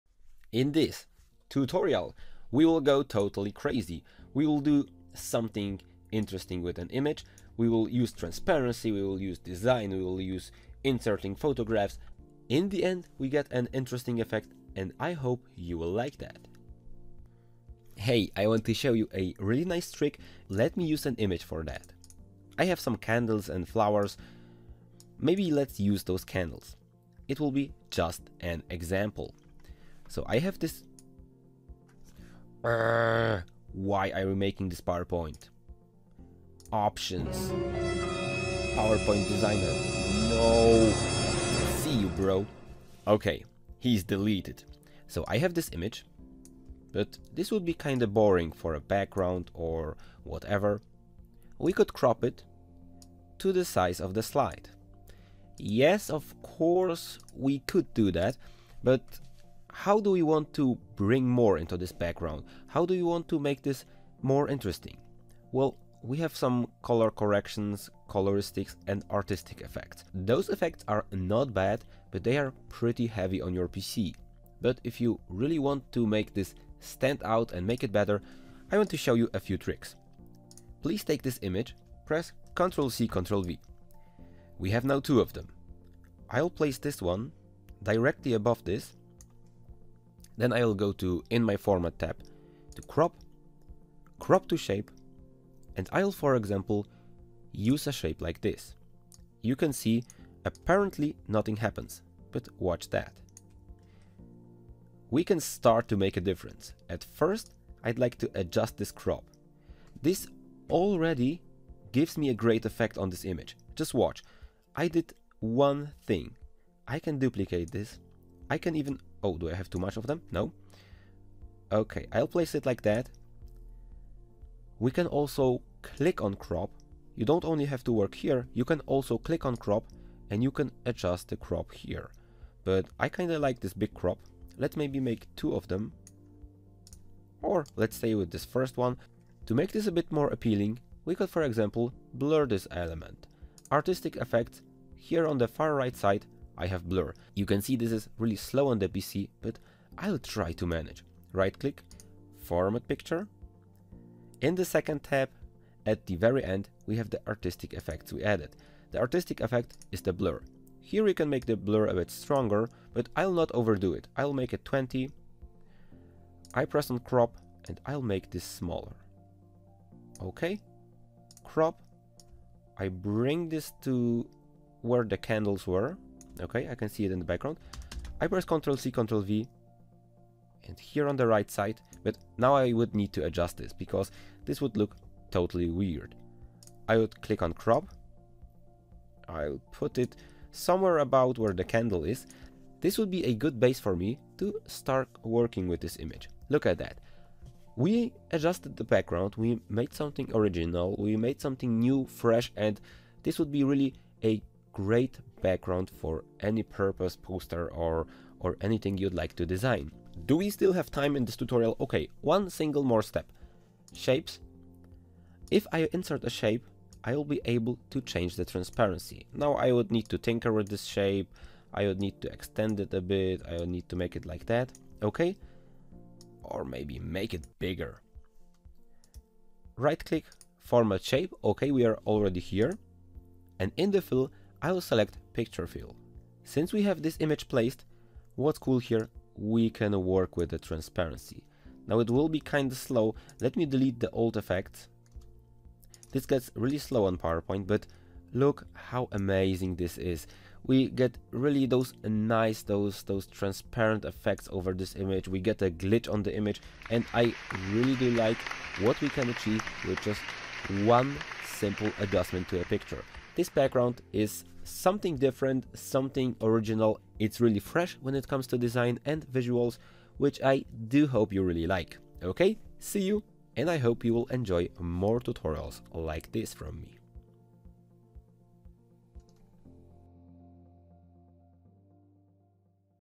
PowerPoint Image background - Chop up Images for Artictic Effects ✔
Get more fun in PPT with my premium course, it's worth it!

So here we are, having an image background and wanting to do something flashy with it, perhaps reveal certain parts of it, add articstic effects or just adding a bit of flavour with color changes. All that is possible if we duplicate the image in powerpoint, crop it to the right amount and then work with shapes or crops within those small areas. The duplicated item can be adjusted while the background remains the same. Easy peasy!